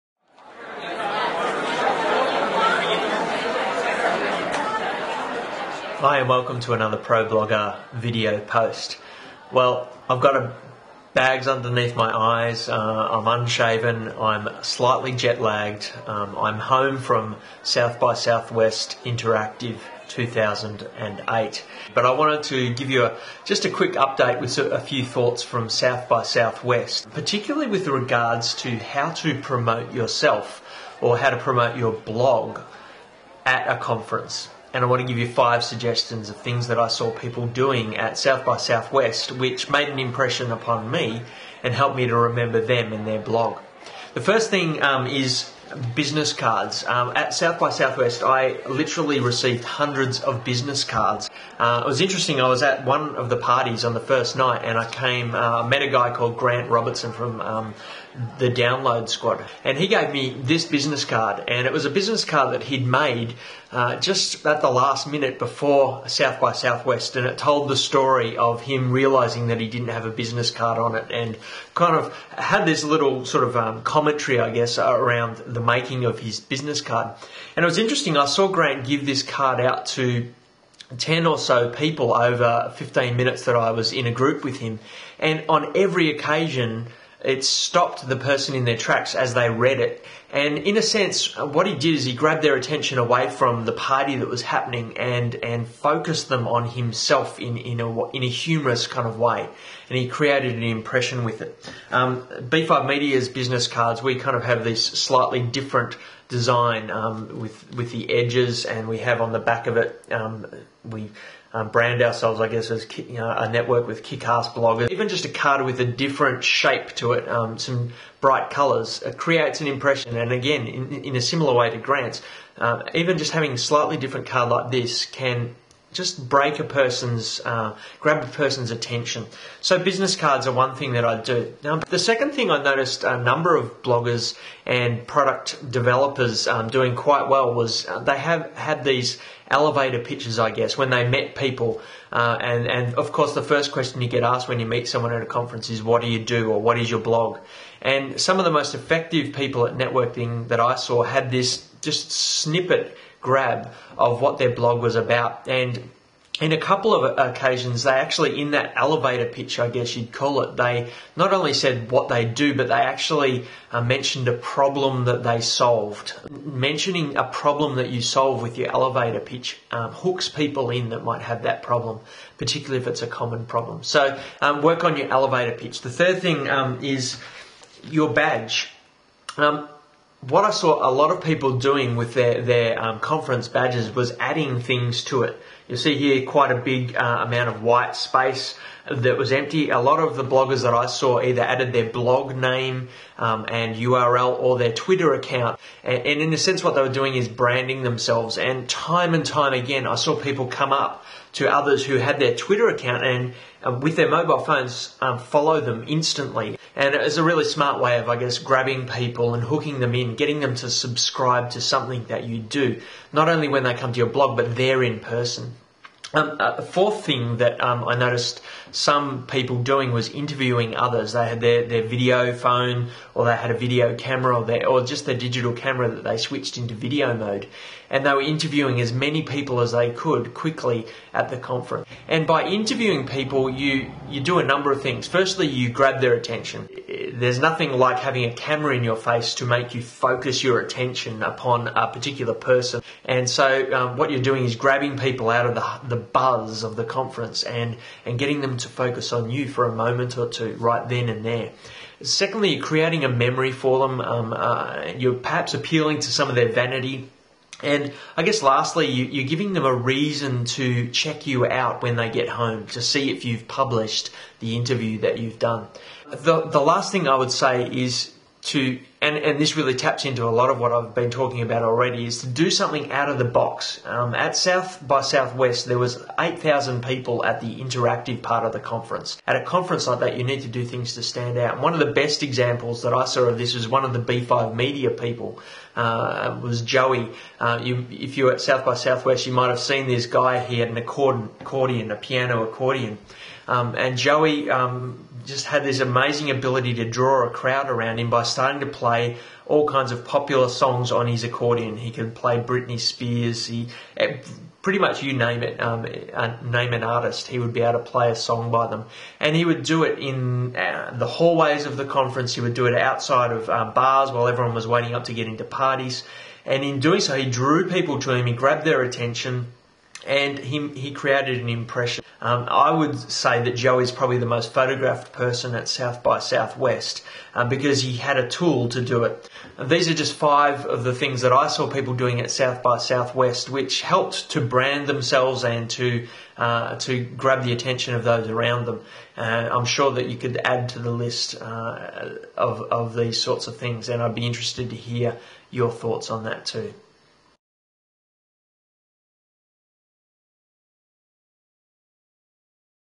How to Promote Yourself (and Your Blog) at a Conference
- conferences are an ideal situation to promote yourself and find new readers for your blog.